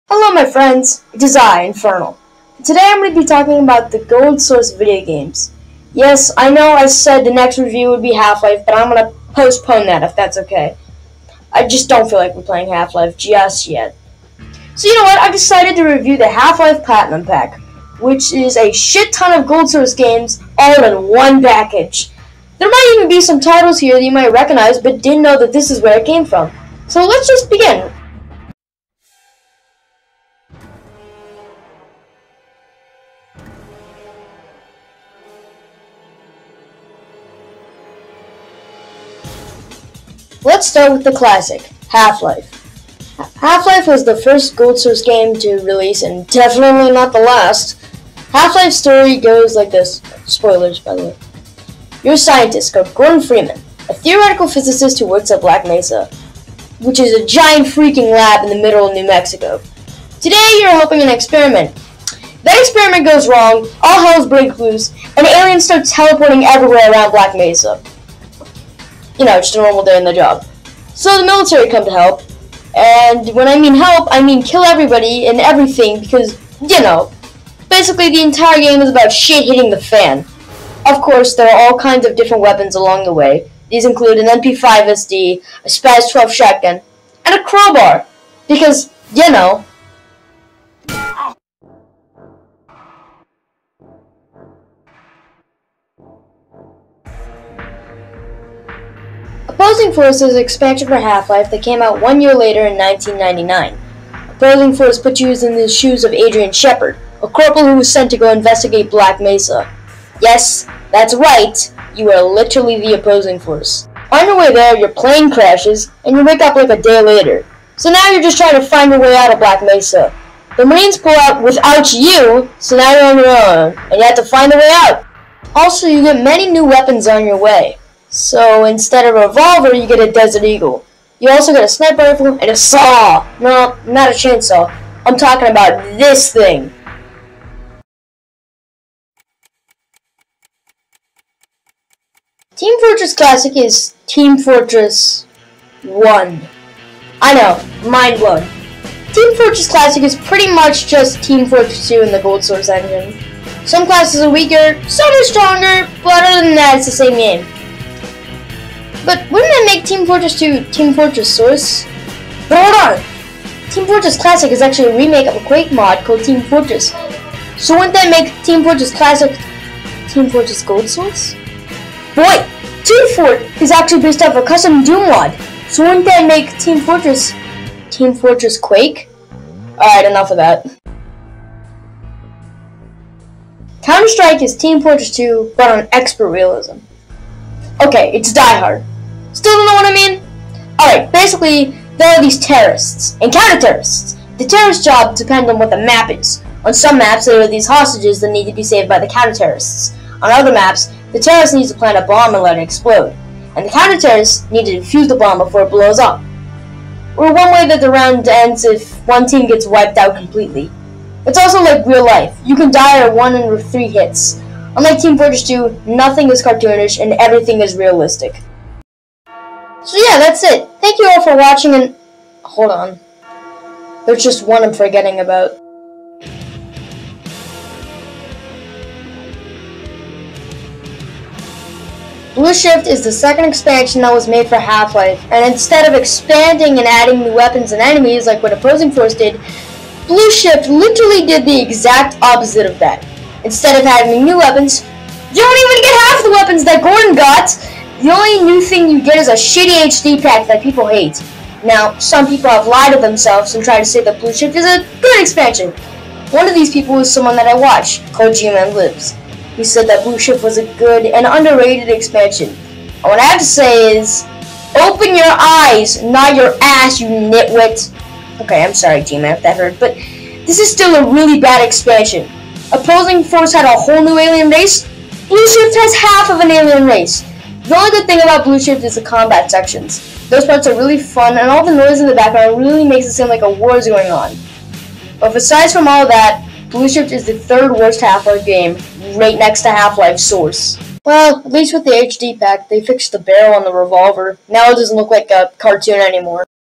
Half-Life: Platinum - MVGT9K
In this video Infernale reviews the Half-Life: Platinum pack.

sry Gmanlives :(